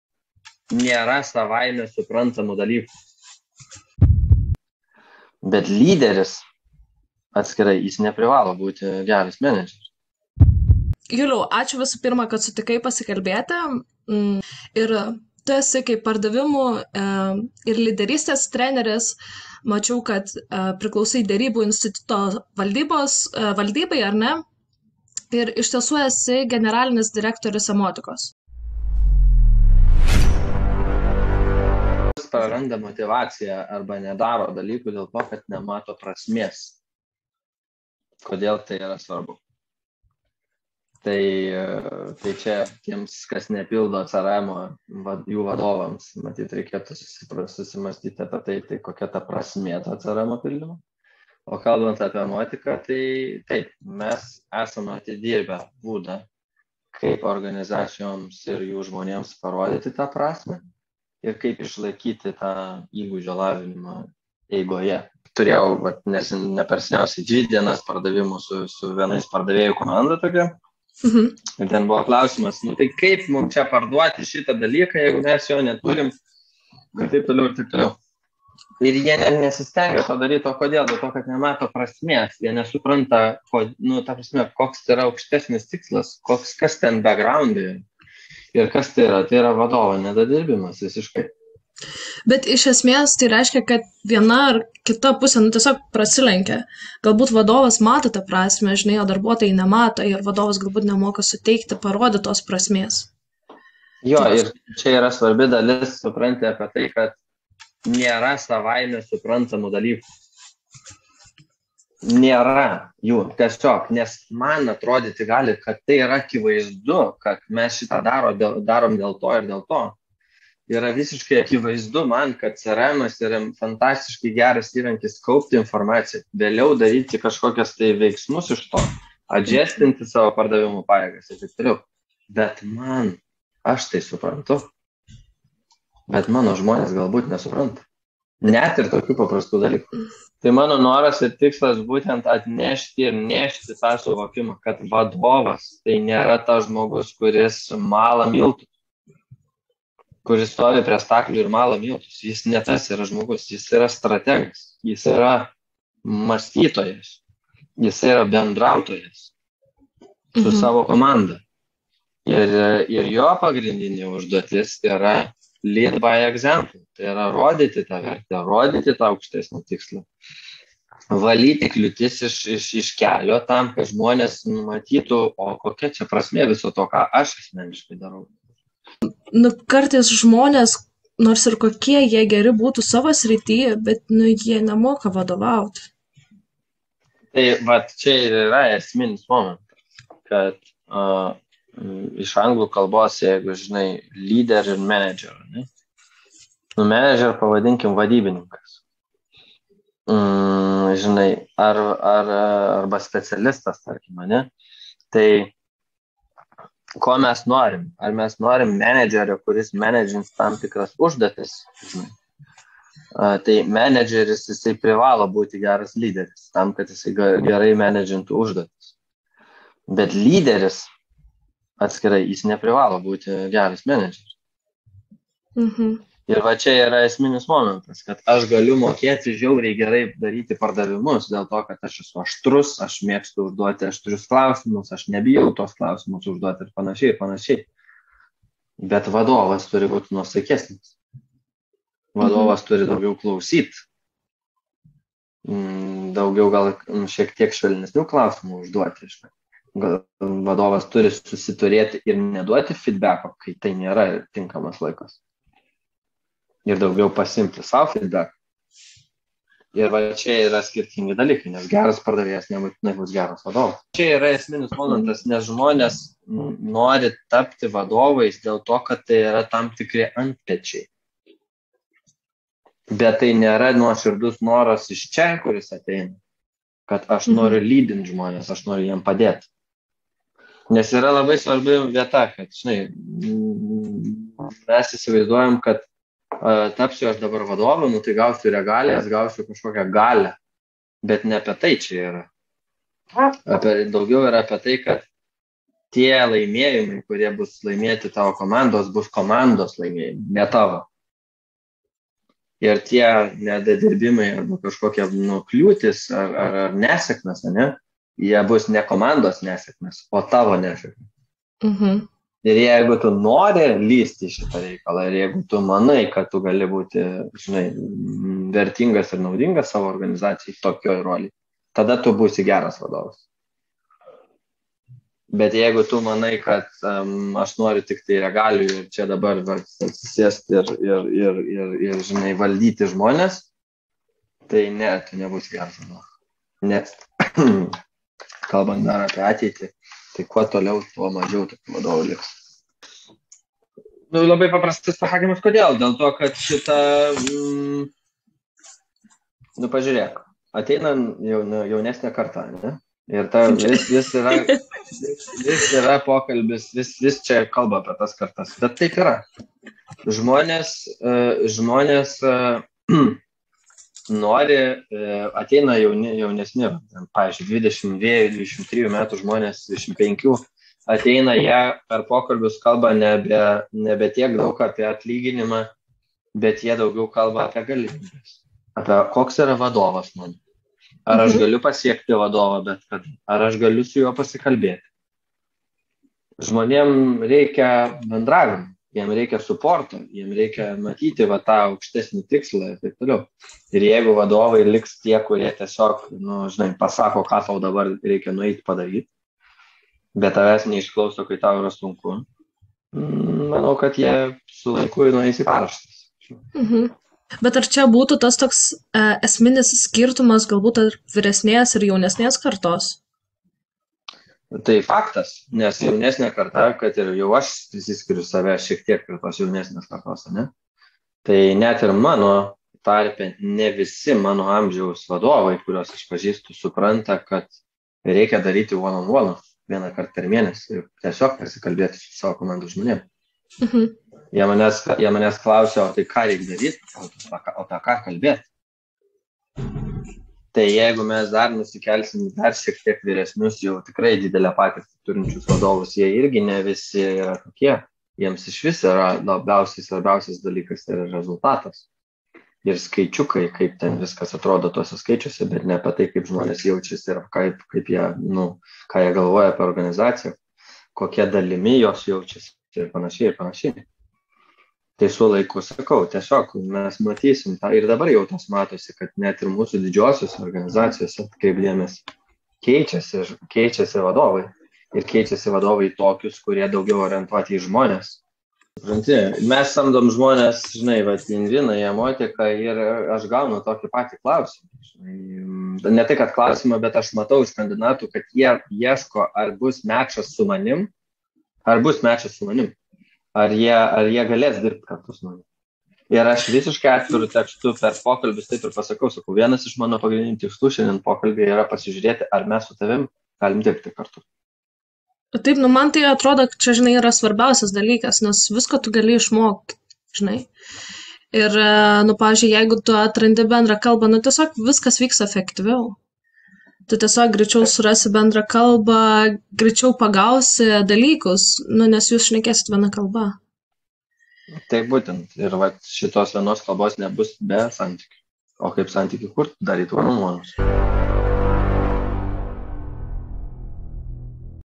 Julius Jankauskas: žmogus praranda motyvaciją, nes neranda prasmės
Julius Jankauskas - „Emotika“ generalinis direktorius, INWIN derybų instituto valdybos pirmininkas, pardavimų ir lyderystės treneris.